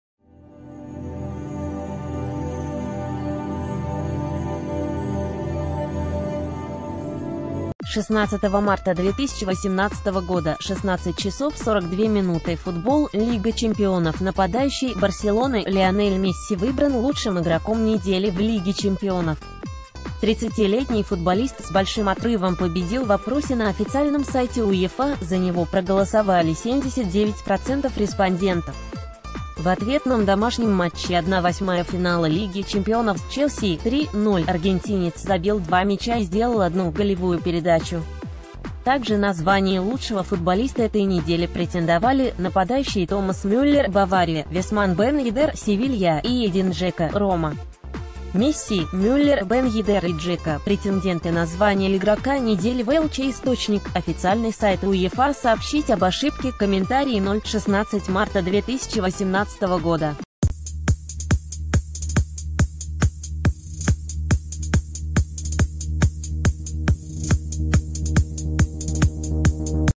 Последние новости | Месси признан игроком недели в Лиге чемпионов
16 марта 2018, 16:42 Футбол / Лига чемпионов Нападающий «Барселоны» Лионель Месси выбран лучшим игроком недели в Лиге чемпионов. 30-летний футболист с большим отрывом победил в опросе на официальном сайте УЕФА — за него проголосовали 79% респондентов. В ответном домашнем матче 1/8 финала Лиги чемпионов с «Челси» (3:0) аргентинец забил два мяча и сделал одну голевую передачу. Также на звание лучшего футболиста этой недели претендовали: нападающие Томас Мюллер («Бавария»), Виссман Бен-Йеддер («Сев...